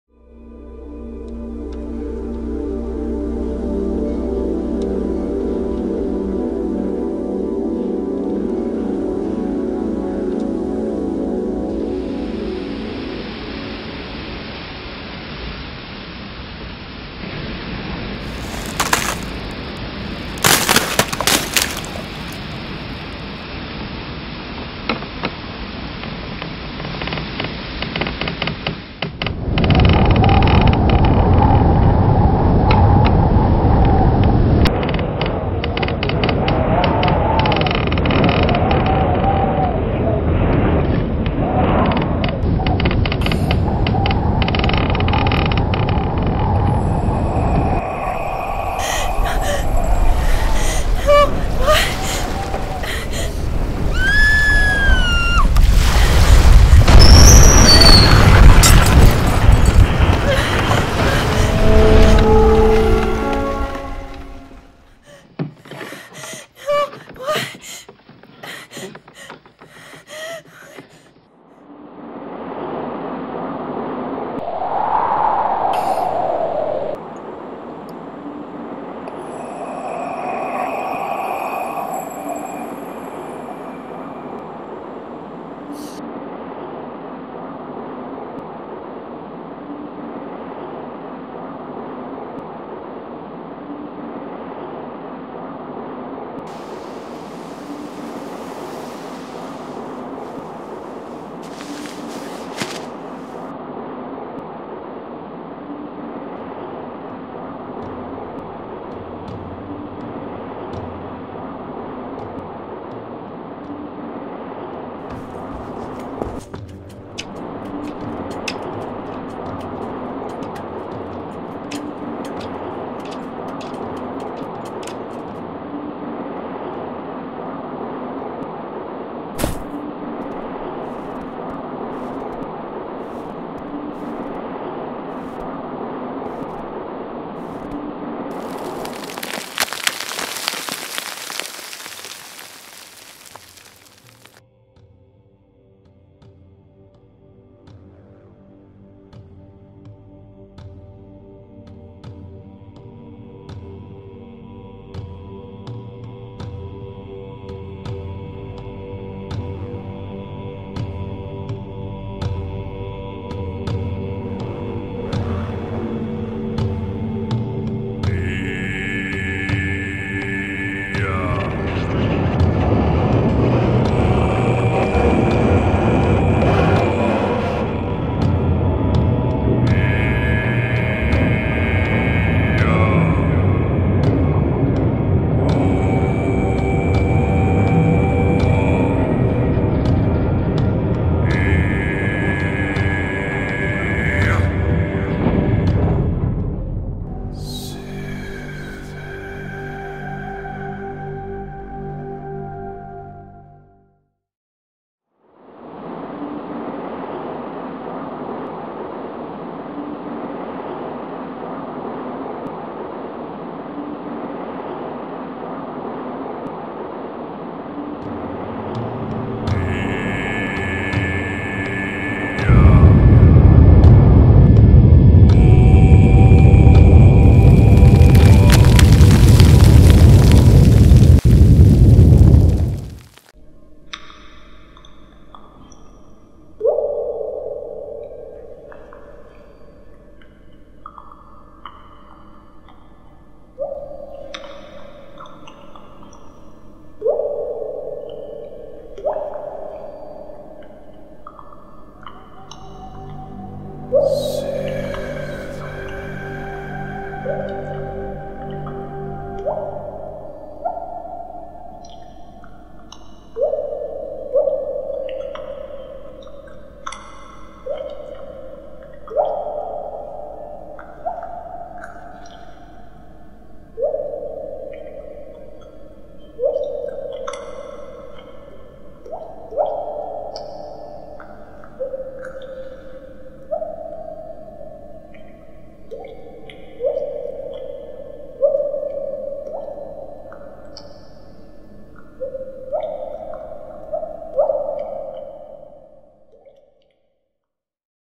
Animatic - Blazing World
A Description of A New World called the Blazing World.
Second Year Illustration University Project.

I made an Animatic Sequence to convey a section of text from one of the earliest Science Fiction pieces written.

Music credited to Syven.

Art work created and owned by William Dunstan. Copyright William Dunstan.